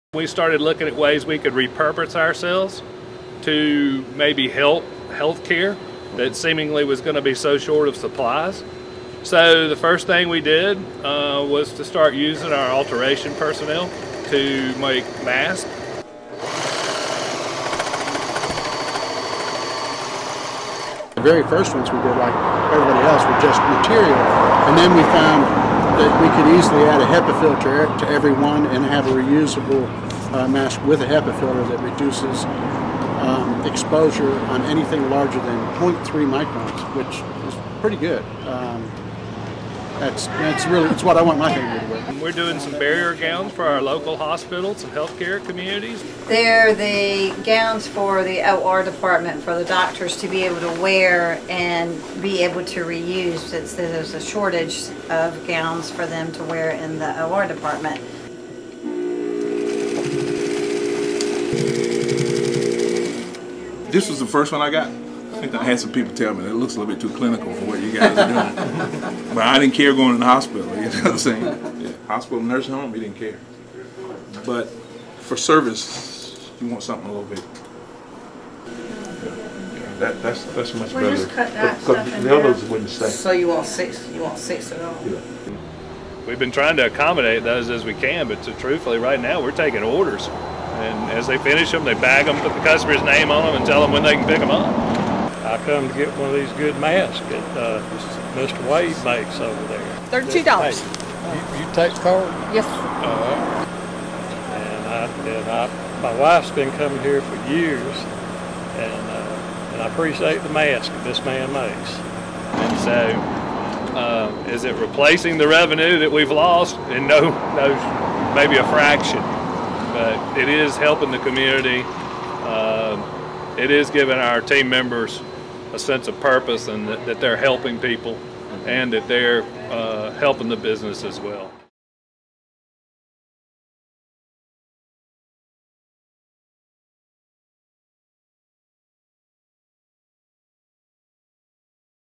Columbus, GA company making PPE to help community, offset business slowdown
The Wade Companies has been a family-owned business since being established in 1939 as Wade Dry Cleaning Company. They're now making Personal Protective Equipment(PPE) to help the community and offset the slowdown caused by the coronavirus pandemic. More from the Ledger-Enquirer: